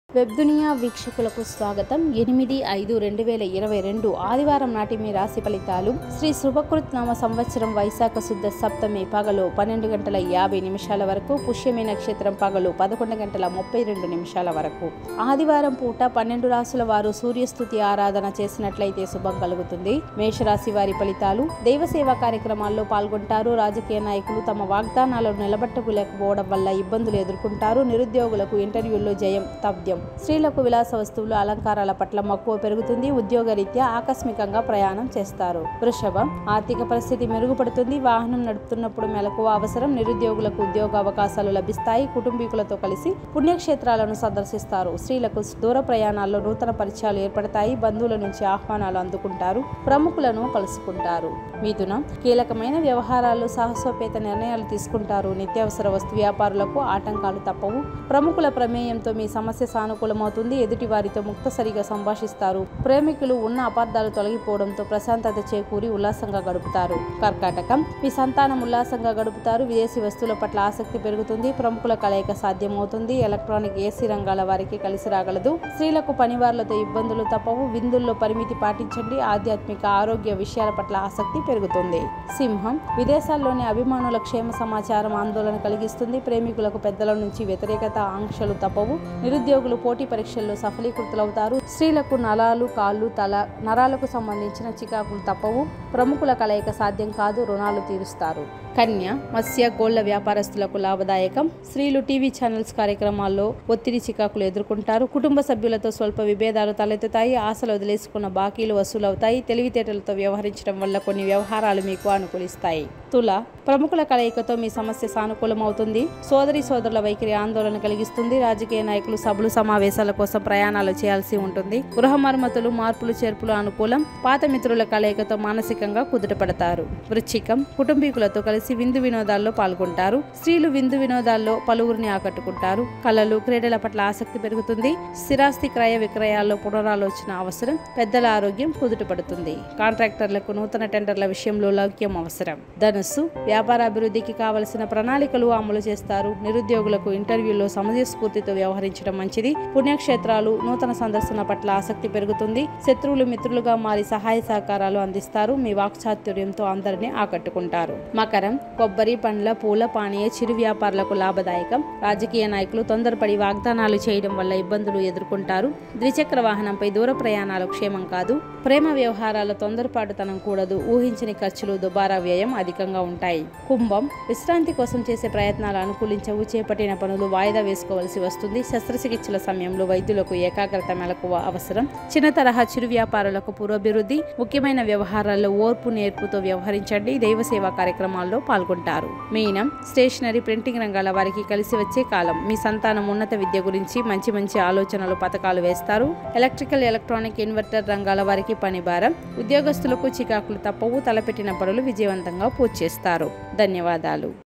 Today Daily Astrology dated on 8th May'2022_ సూర్యస్తుతితో శుభం
08-05-22 ఆదివారం రాశిఫలాలు- సూర్యస్తుతితో శుభం

Today Daily Astrology dated on 8th May'2022

సూర్య స్తుతి ఆరాధించిన శుభం కలుగుతుంది.

మేషం:- దైవ, సేవా, పుణ్యకార్యాలలో నిమగ్నమవుతారు. రాజకీయ నాయకులు తమ వాగ్దానాలను నిలబెట్టుకోలేక పోవడం వల్ల ఇబ్బందులను ఎదుర్కొంటారు. నిరుద్యోగులకు ఇంటర్వ్యూలలో జయం పొందుతారు. స్త్రీలకు విలాసవస్తువులు, అలంకారాల పట్ల మక్కువ పెరుగుతుంది. ఉద్యోగ రీత్యా ఆకస్మికంగా ప్రయాణం చేస్తారు.